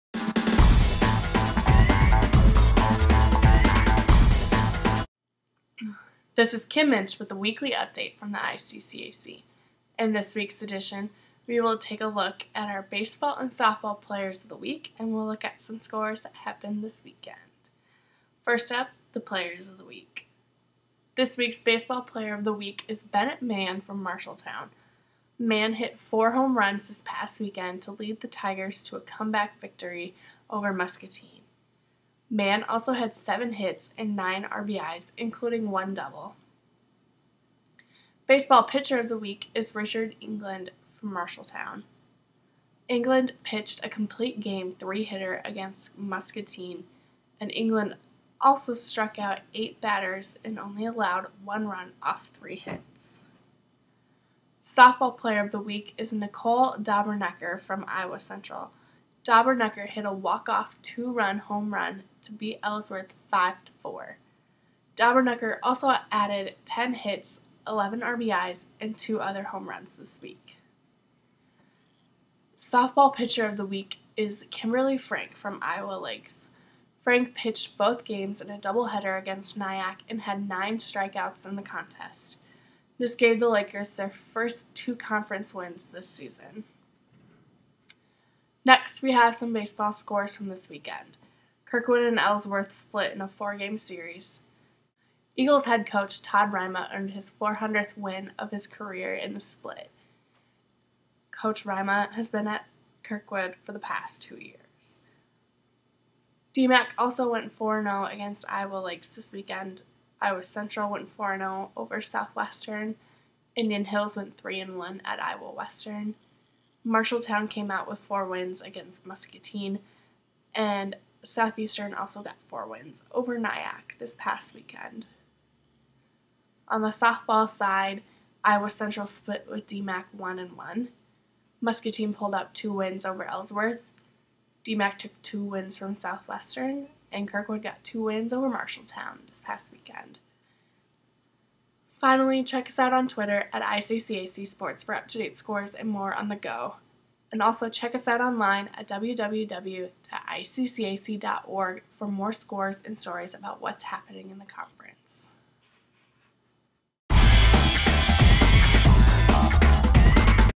ICCAC Weekly Update 4/8/14
In this week's edition, we will be covering the athletes of the week and we'll take a look at some baseball and softball scores from the weekend.